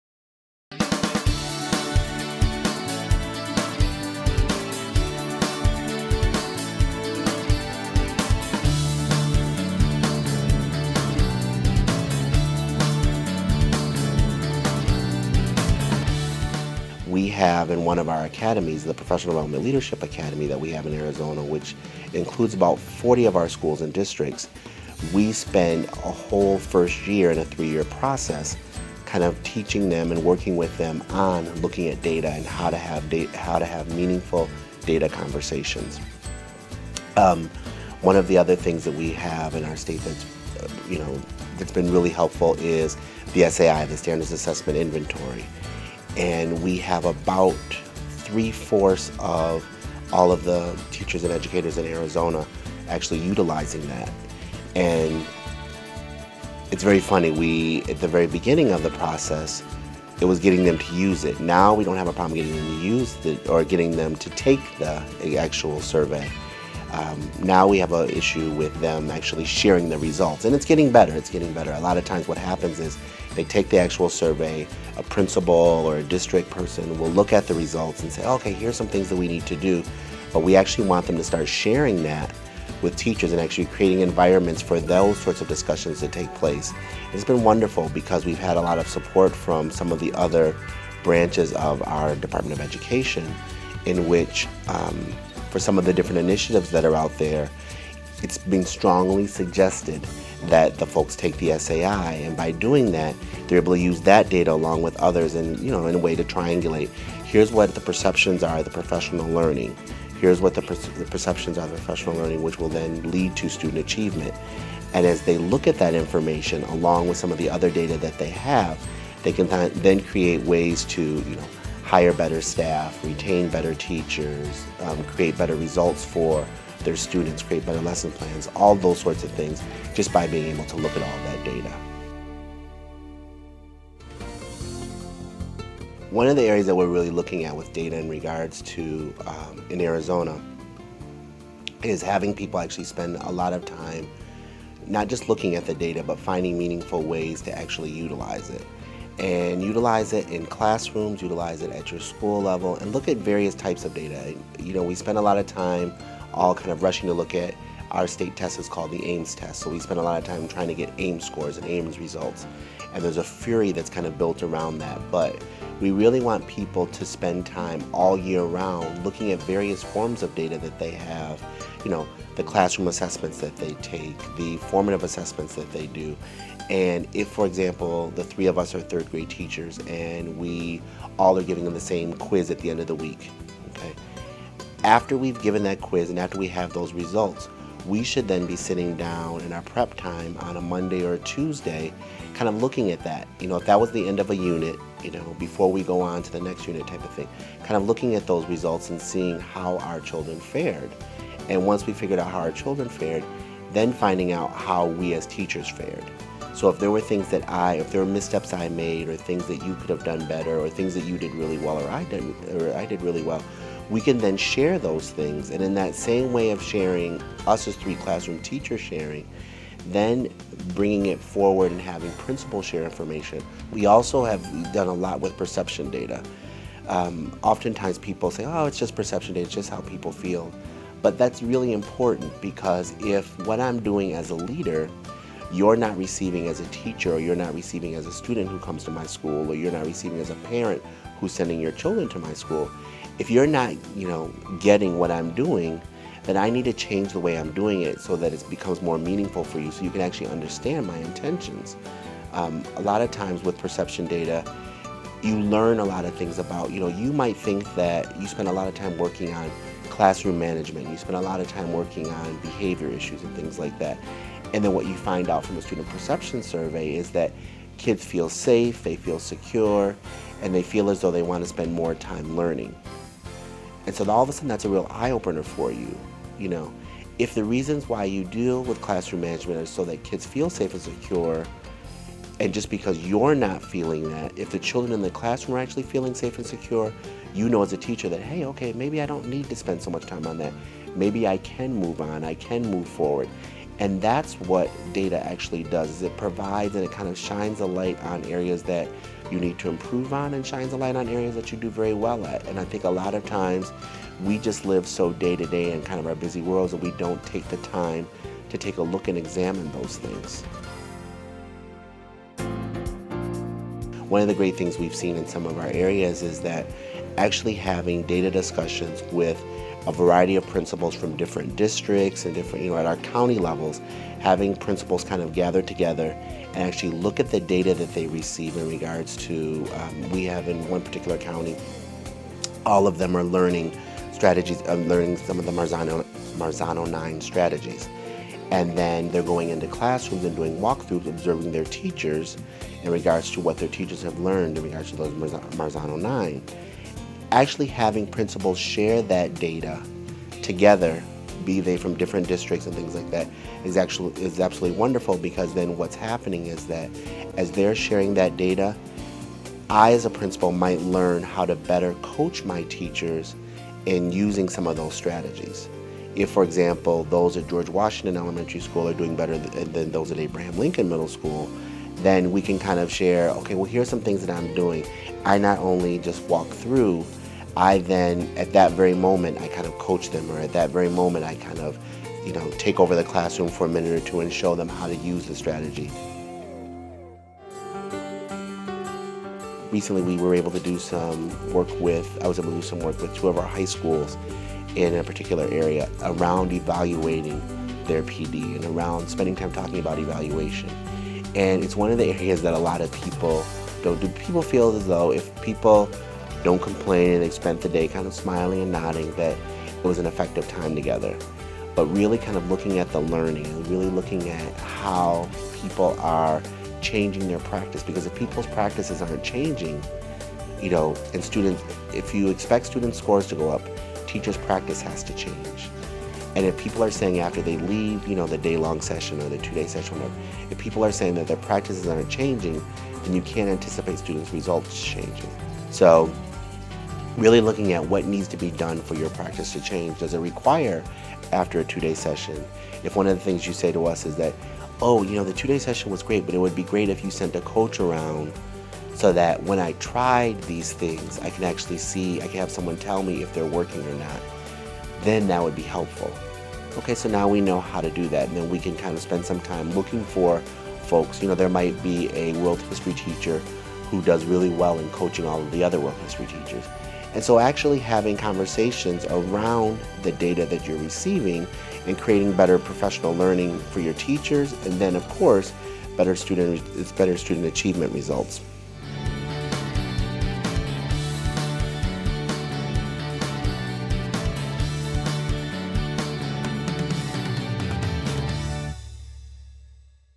Data (From the 2011 standards)
Eric Brooks, education program specialist, discuss the importance and use of data. Professional learning that increases educator effectiveness and results for all students uses a variety of sources and types of student, educator, and system data to plan, assess, and evaluate professional learning.

Learn more about the latest Standards for Professional Learning